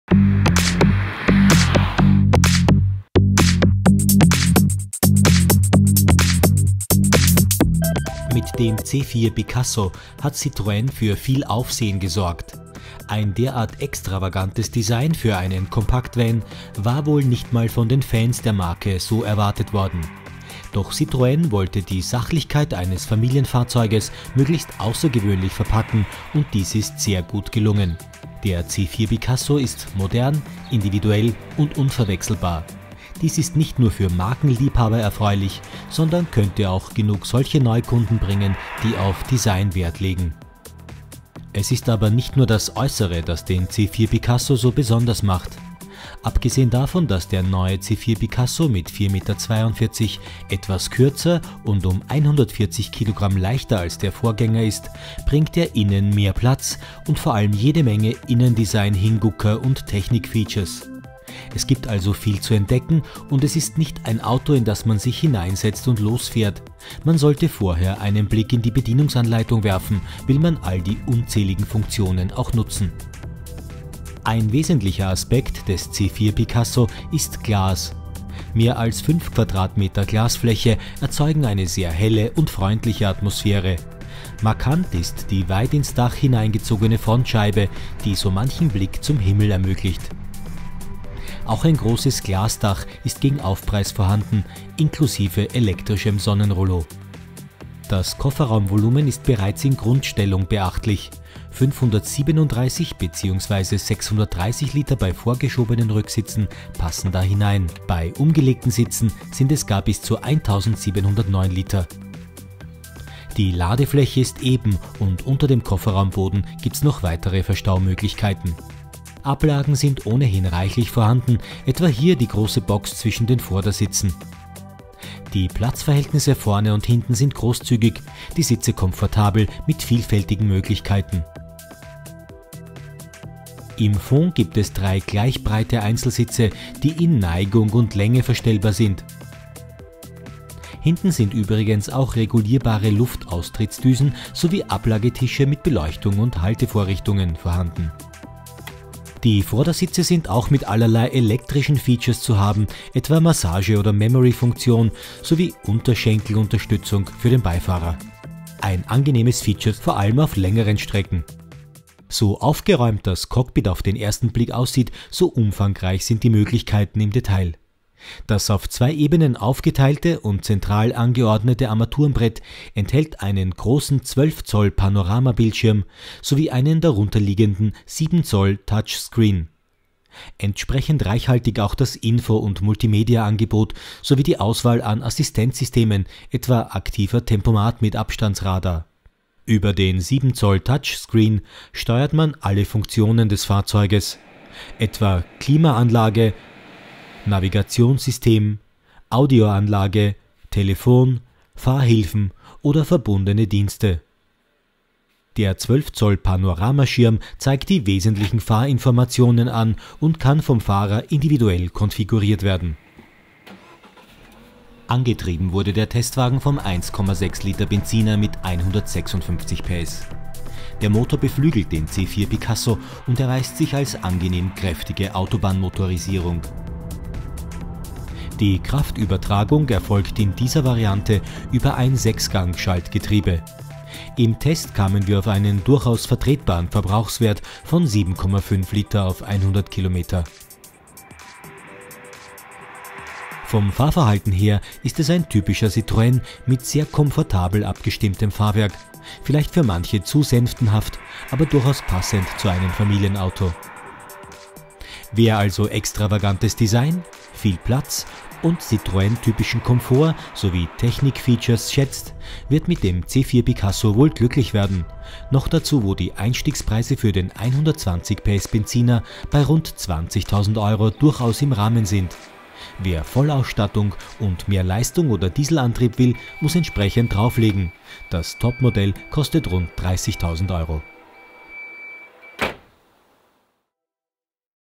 Citroen C4 Picasso THP 155
Mit dem Citroen C4 Picasso THP 155 hat der französische Hersteller einen geräumigen Kompaktvan in starker Benzinmotorisierung im Programm. Das 1.6-Liter-Turboaggregat leistet 156 PS und ist gemeinsam mit einem Sechsgang-Automatikgetriebe für den Vortrieb im Testwagen zuständig. Alles dazu im Video